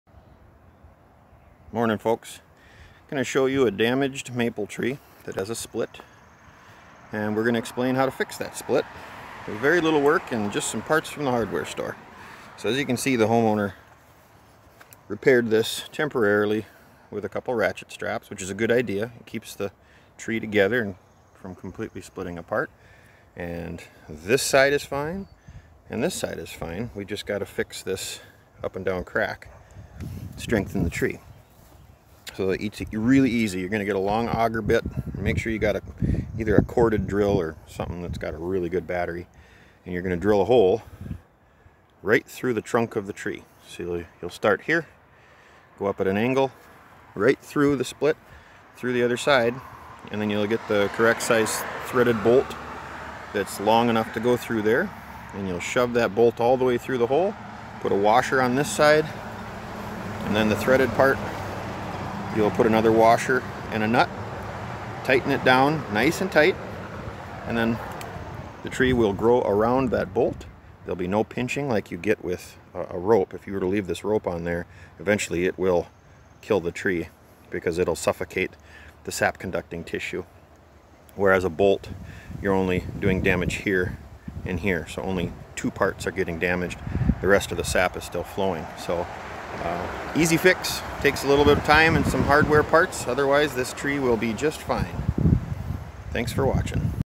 I will show you how to repair a splitting tree without girdling it
How to repair a splitting tree without girdling it.